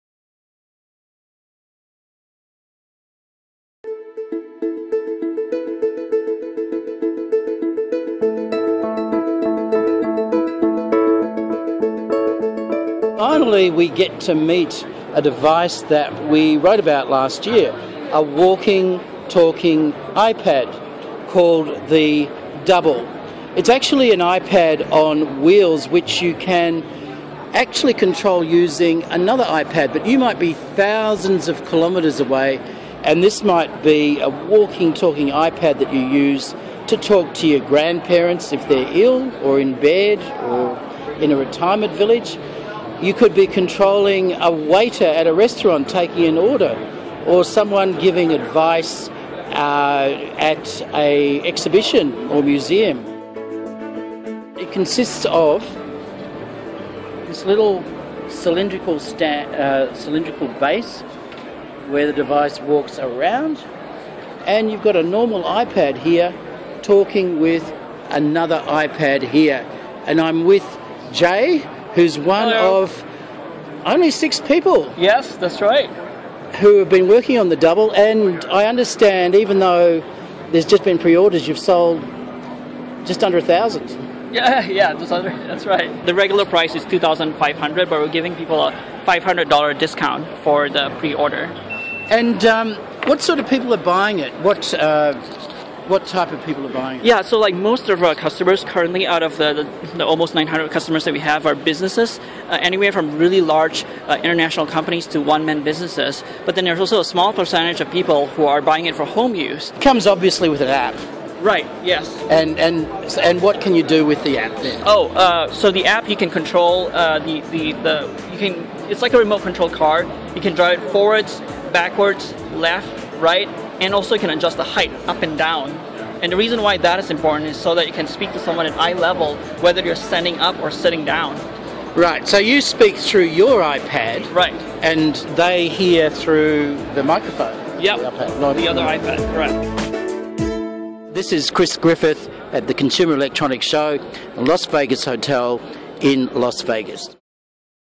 CES 2013 NEW!! The walking, talking iPad - Remote Presence Robotics
Chris Griffith reviews the latest in remote presence robotics - technology that allows an iPad to walk and then talk across the world.